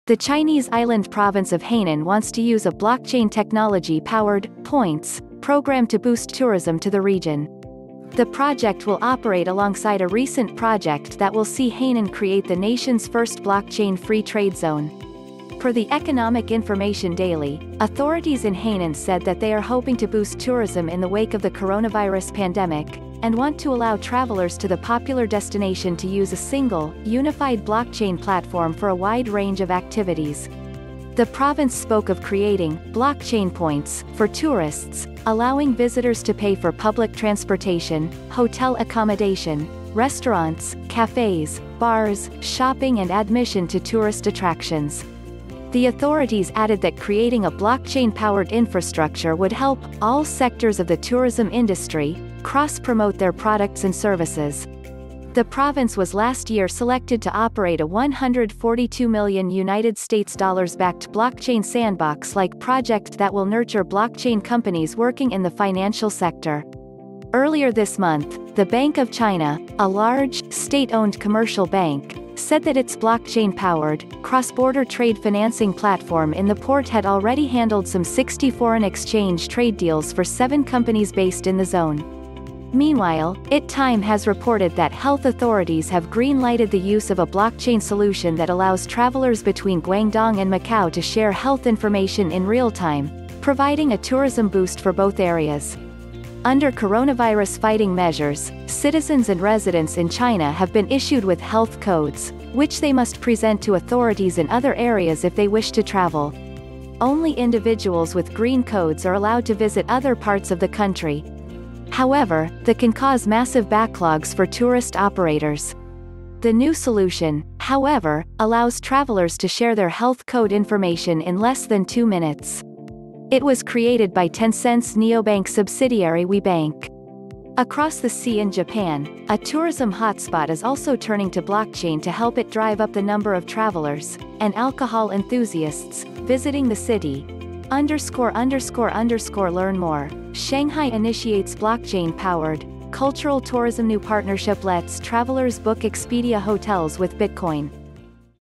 China Using Blockchain Power to Boost Post COVID 19 Tourism
The Chinese island province of Hainan wants to use a blockchain technology-powered “points” program to boost tourism to the region. The project will operate alongside a recent project that will see Hainan create the nation’s first  blockchain free trade zone. Per the Economic Information Daily, authorities in Hainan said that they are hoping to boost tourism in the wake of the coronavirus pandemic, and want to allow travelers to the popular destination to use a single, unified blockchain platform for a wide range of activities. The province spoke of creating “blockchain points” for tourists, allowing visitors to pay for public transportation, hotel accommodation, restaurants, cafes, bars, shopping and admission to tourist attractions. The authorities added that creating a blockchain-powered infrastructure would help “all sectors of the tourism industry” cross-promote their products and services. The province was last year selected to operate a USD 142m-backed blockchain sandbox-like project that will nurture blockchain companies working in the financial sector. Earlier this month, the Bank of China, a large, state-owned commercial bank, said that its blockchain-powered, cross-border trade financing platform in the port had already handled some 60 foreign exchange trade deals for seven companies based in the zone. Meanwhile, IT Time has reported that health authorities have green-lighted the use of a blockchain solution that allows travelers between Guangdong and Macao to share health information in real-time – providing a tourism boost for both areas. Under coronavirus-fighting measures, citizens and residents in China have been issued with health codes – which they must present to authorities in other areas if they wish to travel. Only individuals with green codes are allowed to visit other parts of the country. However, the can cause massive backlogs for tourist operators. The new solution, however, allows travelers to share their health code information in less than two minutes. It was created by Tencent’s neobank subsidiary WeBank. Across the sea in Japan, a tourism hotspot is also turning to blockchain to help it drive up the number of travelers – and alcohol enthusiasts – visiting the city.___Learn more: Shanghai Initiates Blockchain-powered 'Cultural Tourism'New Partnership Lets Travelers Book Expedia Hotels with Bitcoin